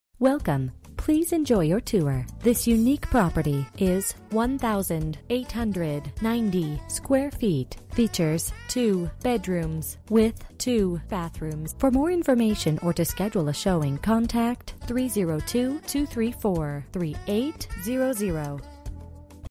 Residential for sale - 003 RUTLAND AVE, MIDDLETOWN, DE 19709
Whether you're looking for a detached single-family home large enough to raise a growing family, an intelligently designed attached carriage home with the versatility to offer a first-floor owners bedroom suite, or a modern townhome with lots of living space, it's all here at Ponds of Odessa. Finally, a community where value-conscious homebuyers will discover a wide range of inviting home designs that are architecturally stimulating, amazingly affordable, immensely livable, superbly energy efficient and built to the same level of signature craftsmanship found in every Benchmark Builders home. Regardless of your choice, you can expect a comprehensive list of standard features, a beautifully designed setting with plenty of open space, the convenience of public water and sewer and low property taxes. Located in the esteemed Appoquinimink School District, these homes truly are everything but average.

Bedrooms: 2
Bathrooms: 2 bathrooms
Square Feet: 1890
Price: $287,900